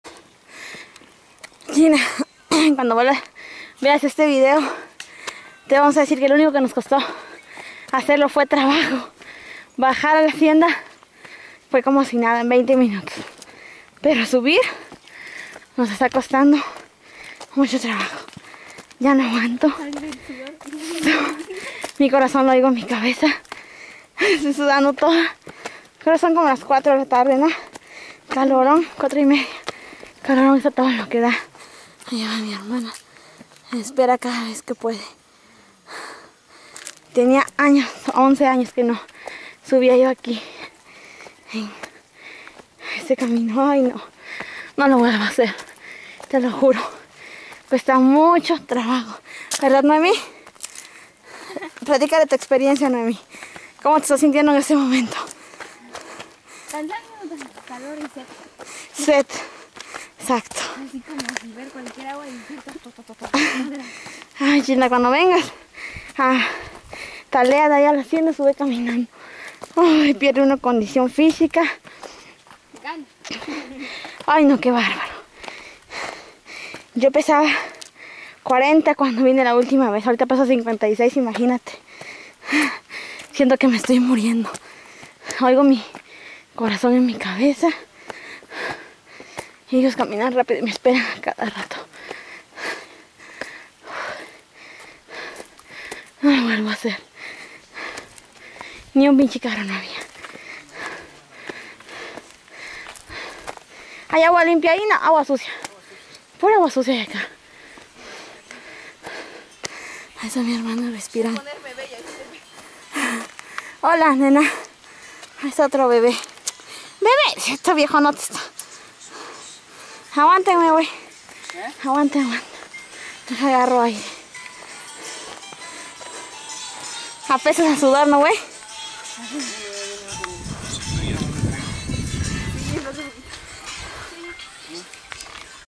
ex hda sta getrudis
de regreso  hacia  Talea  de castro, depues  de un maravilloso rrecorrido  por la ex hacienda  santa getrudis.  estab muy  pesada  la subida, al menos  para  mi!!!,,, ya que tenia  mas de 11  años  sin  caminar  tanto,, pero  valió  la pena ya que  nos  la pasamos muy bien.